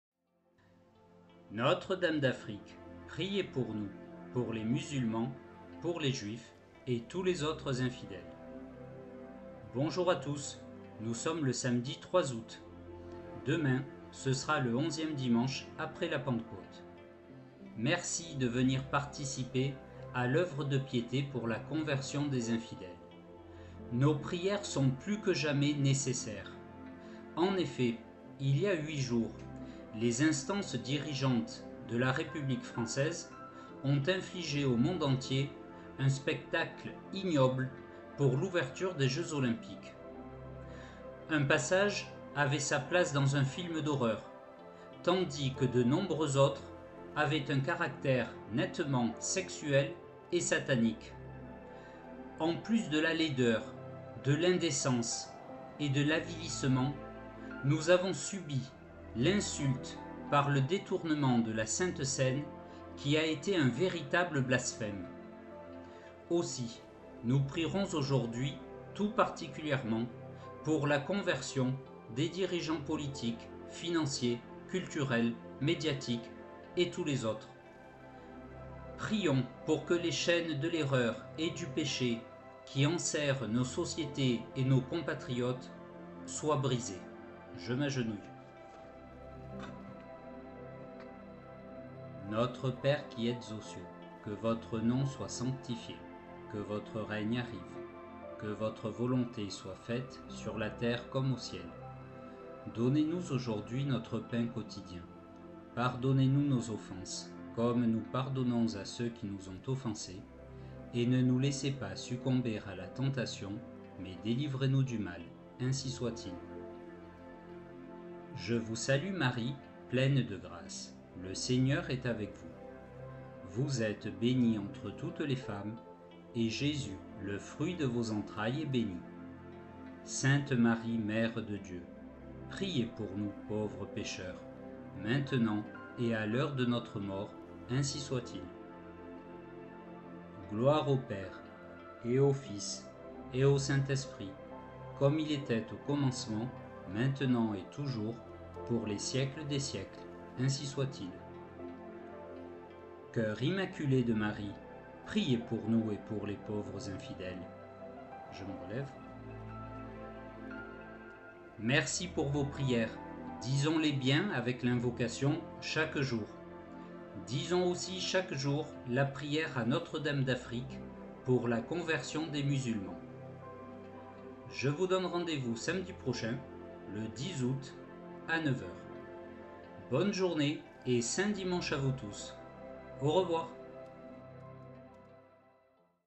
Prières pour la conversion des infidèles, 3 août 2024
Notre-Dame d'Afrique, priez pour nous, pour les musulmans, pour les juifs et tous les autres infidèles.

Prières pour convertir les dirigeants satanistes, francs-maçons... qui nous ont infligé un spectacle horrible et blasphématoire à l'occasion de l'ouverture des jeux olympiques.

Participons à l'œuvre de piété pour la conversion des infidèles, chaque samedi, à 9 heures. Nous prions ensemble pour que tous les hommes soient réunis dans la même foi, la même espérance et le même amour de Notre-Seigneur Jésus-Christ.

À dire tous les jours : Prière à Notre-Dame d'Afrique pour la conversion des musulmans, de Mgr Pavy

00:00 Invocation ; salutations à vous tous
00:17 Nécessité de prier suite au blasphème olympique
01:31 Prières et invocation pour la conversion des infidèles
02:41 Remerciements et rendez-vous